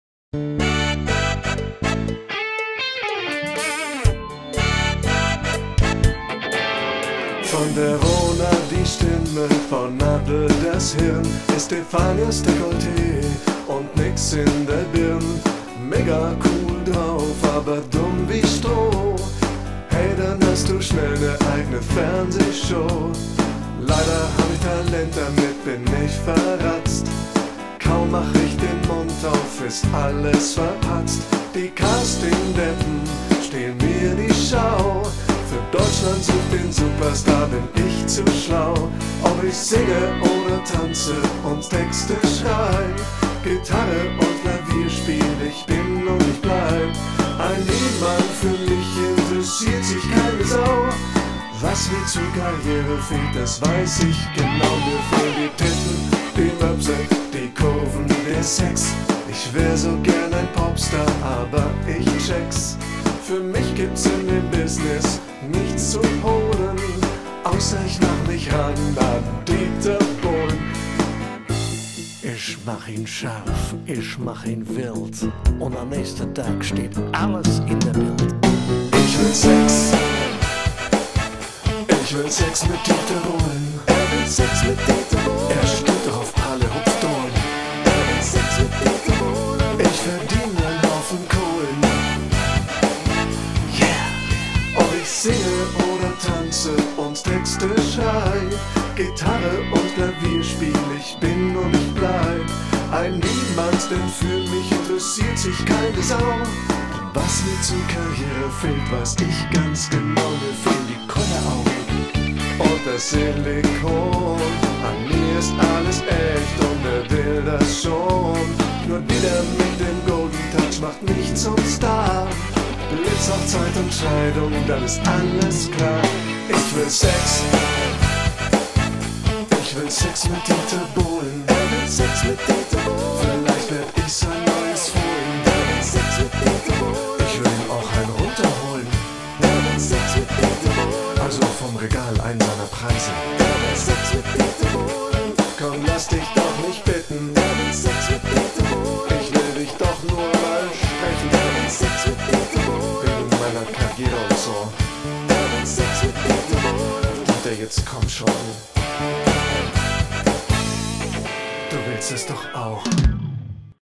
Castingshows Sartiresong
Bist du ein verkanntes Musik-Genie? Zu schlau für DSDS? Zu alt für Castingsshows? - Warum Talent einsetzen? Berühmt werden kann so einfach sein: Sex mit Dieter Bohlen. ;-)